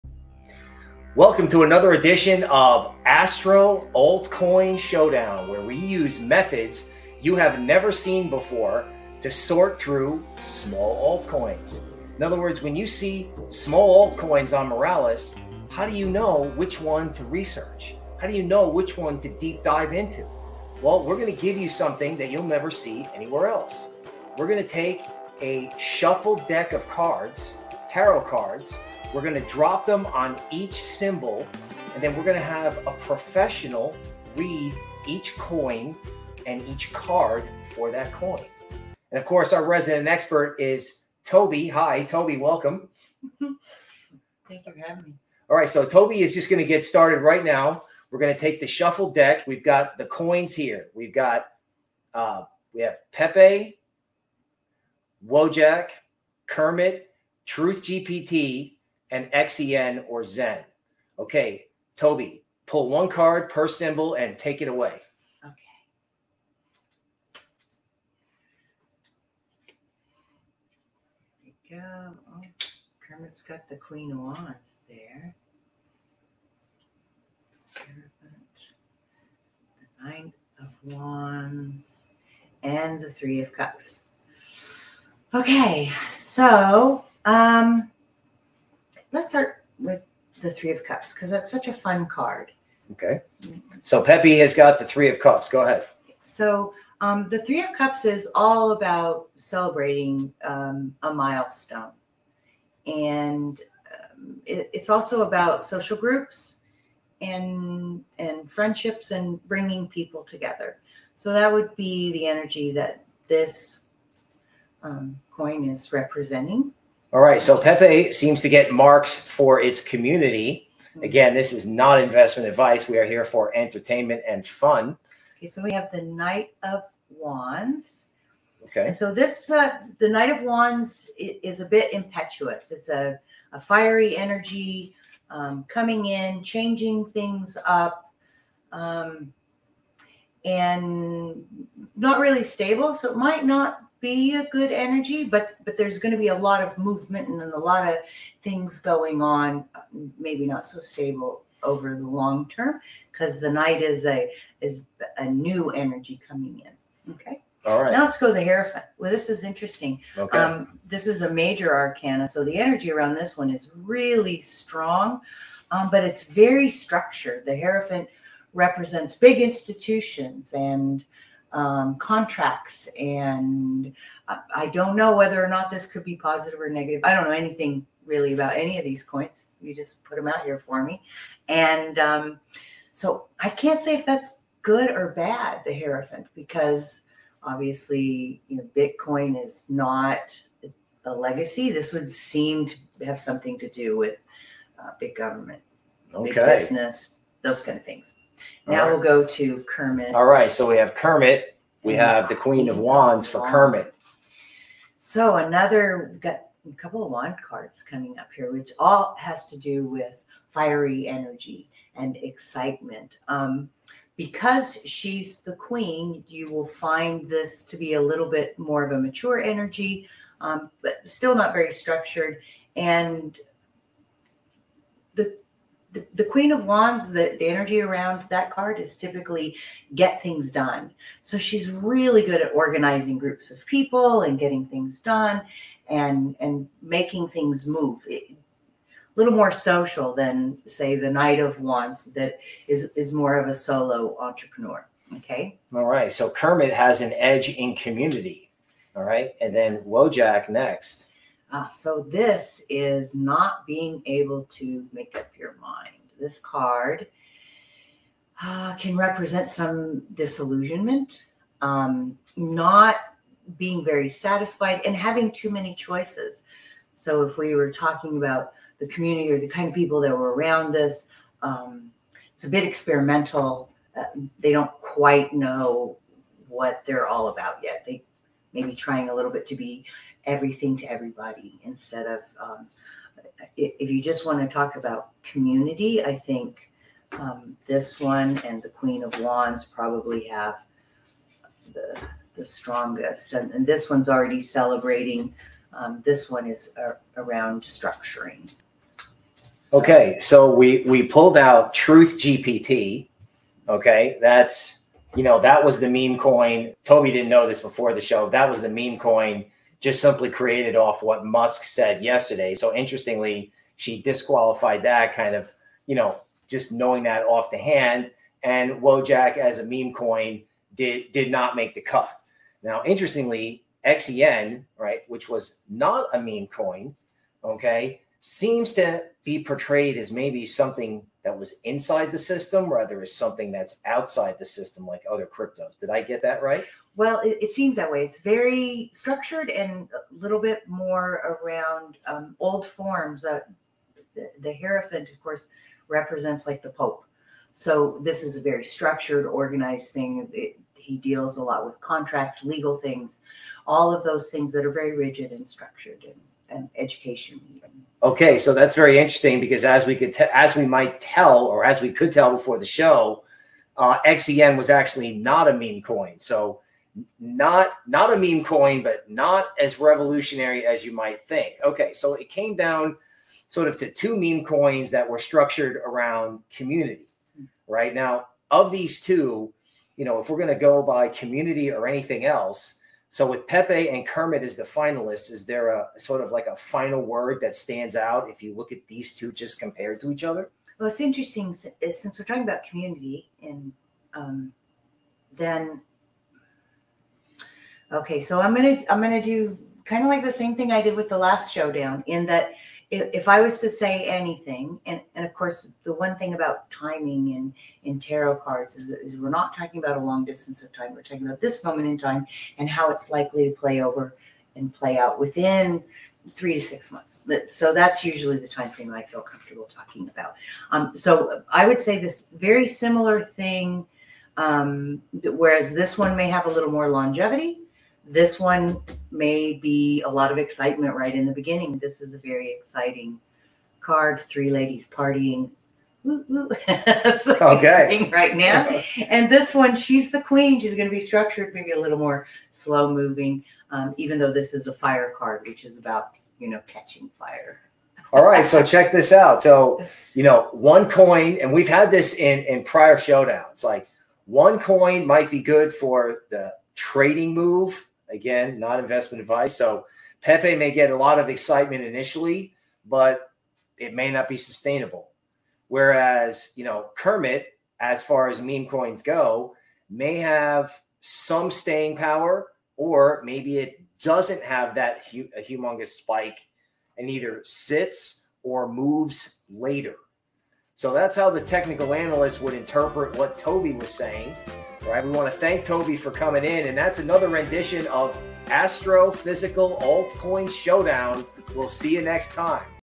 Mystical Altcoin Showdown 💫🔮
In this video, we're harnessing Gann's secrets, astrology's cosmic insights, and Tarot's mystic wisdom to unlock the hidden potential of speculative altcoins.  Watch this video to find out more!

●▬▬▬▬🕰Timestamps🕰▬▬▬▬●

0:00 Crypto Showdown Tarot Card Reading
0:39 Toby introduction
0:50 Altcoins selected for reading
1:02 1st Tarot Card Drawn
1:11 2nd Tarot Card Drawn
1:14 3rd Tarot Card Drawn
1:19 4th Tarot Card Drawn
1:21 5th Tarot Card Drawn
1:24 $PEPE:  5th Tarot Card Reading
2:05 $TRUTH:  4th Tarot Card Reading
2:36 $XEN:  3rd Tarot Card Reading
3:18 $KERMIT:  2nd Tarot Card Reading
4:08 $WOJACK:  1st Tarot Card Reading
4:42 Final Two Altcoins Chosen
4:58 Altcoins disqualified
6:12 Comparing the last 2 remaining altcoins
6:43 Toby's analysis of 2 remaining altcoins
7:28 Notorious Bill's Analysis
8:01 Conclusion

We would like to thank Moralis Money for helping us find these altcoins to do readings

●▬▬▬▬Socials▬▬▬▬●